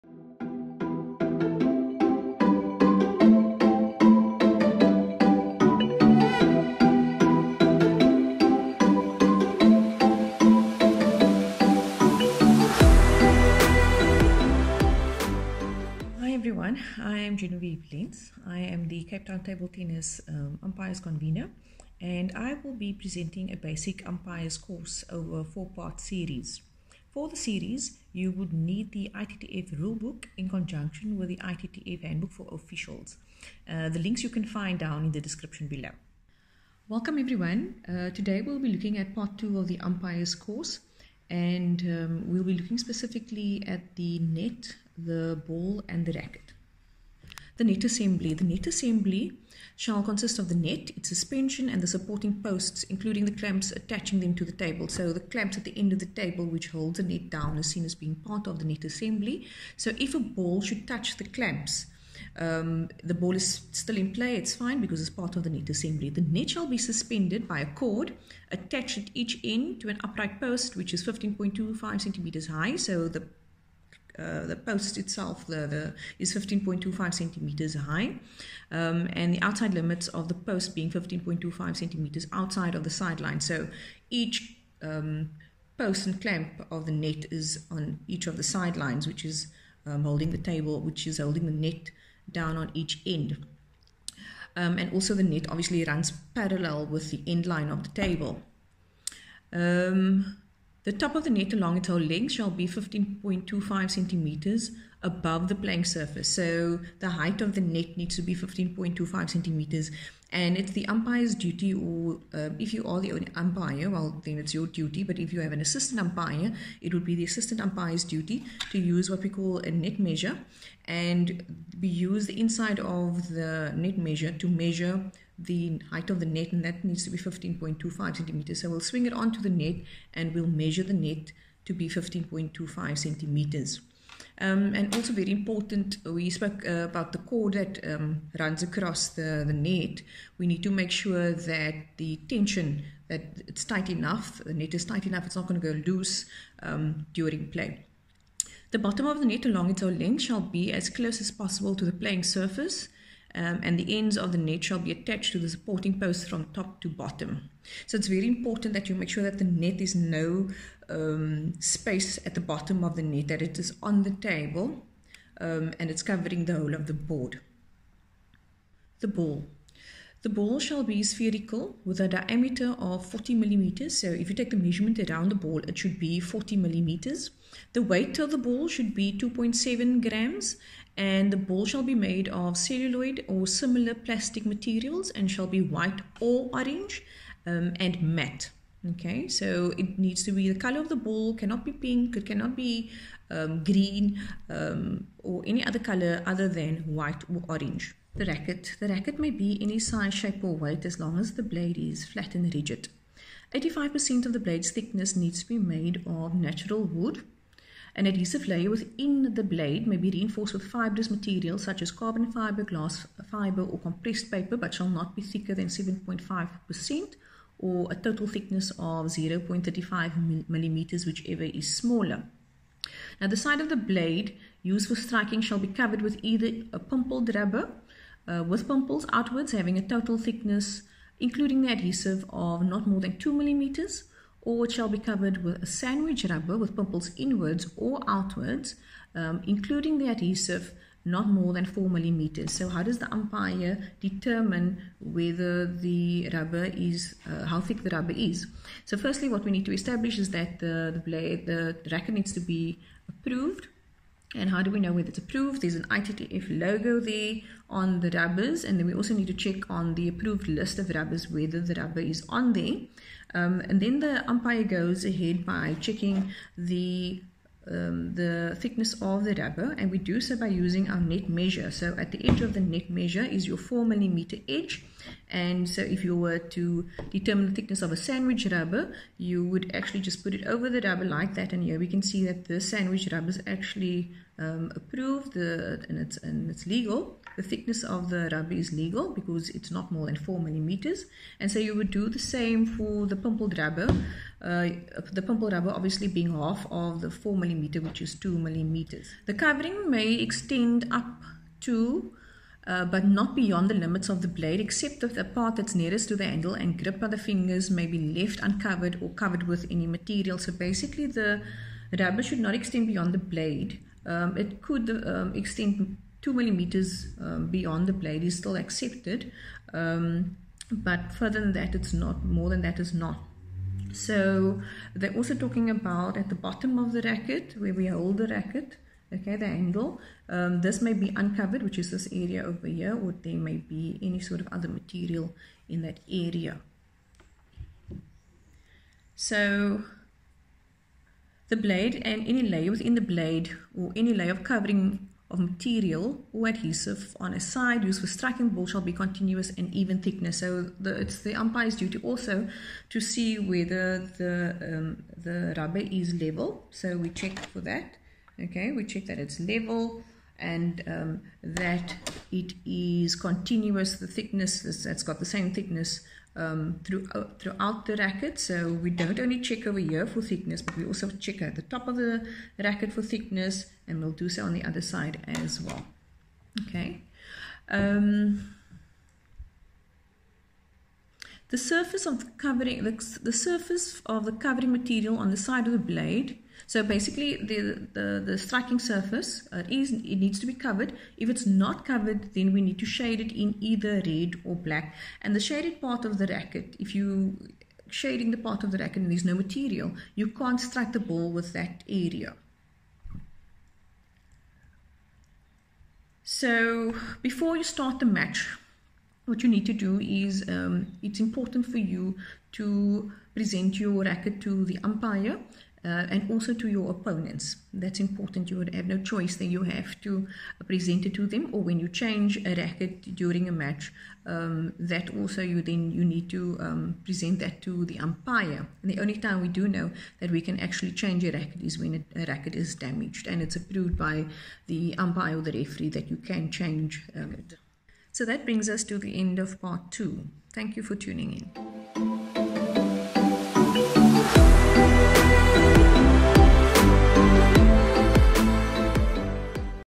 CTTT Level 1 Umpires Course (Basic) : Part 2
Ms Genevieve Lentz : Level 1 Umpire Course Conductor.

Ms Lentz is the Cape Town Table Tennis Umpires Convener and holds the below table tennis qualifications:

International Table Tennis Referee: ITTF IR 17228
International Umpire: ITTF IU 06109

The Level 1 Umpire’s Course is a basic course that covers the Laws of Table Tennis.

The Course is divided into 4 sections:
Video 1: The Table
Video 2: The Net, Ball & Racket
Video 3: Definitions
Video 4: Order of Play, Serving & Receiving Ends and the Expedite System